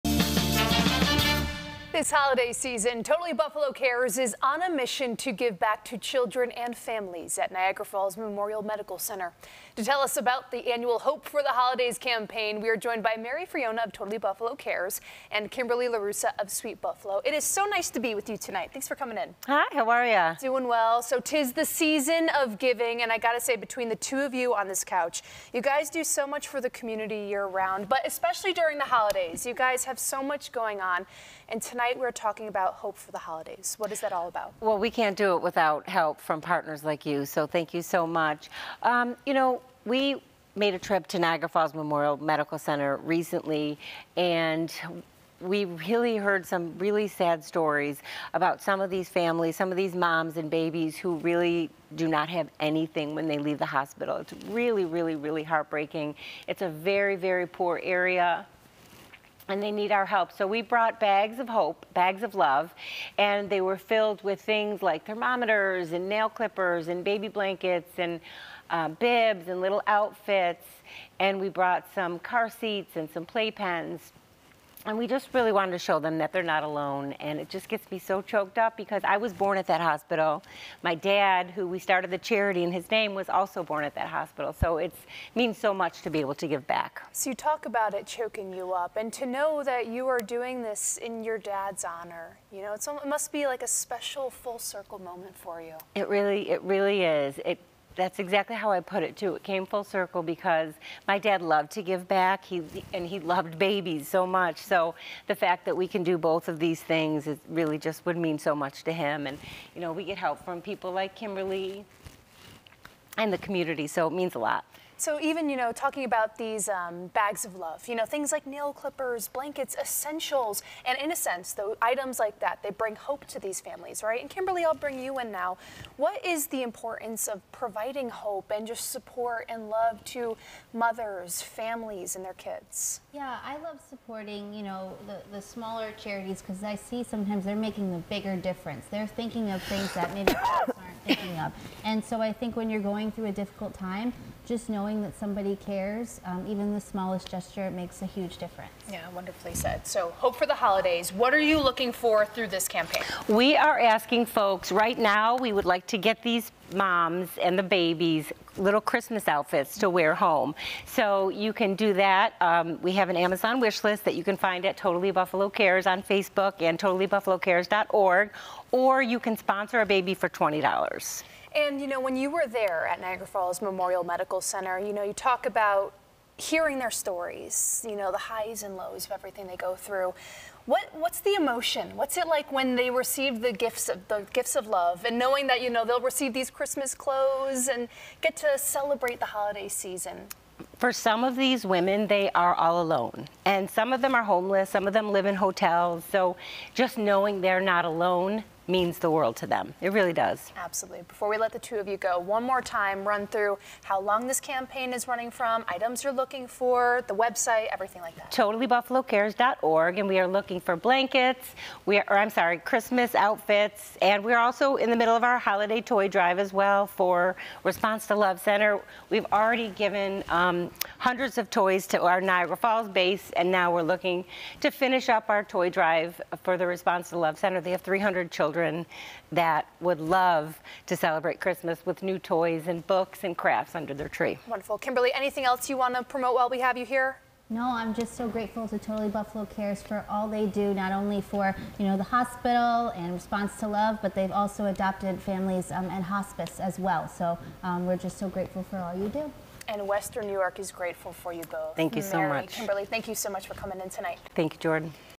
Totally Buffalo Cares seeks gift donations for local hospital babies
Totally Buffalo Cares is asking for your help bringing "Hope for the Holidays" to local babies at Niagara Falls Memorial Medical Center.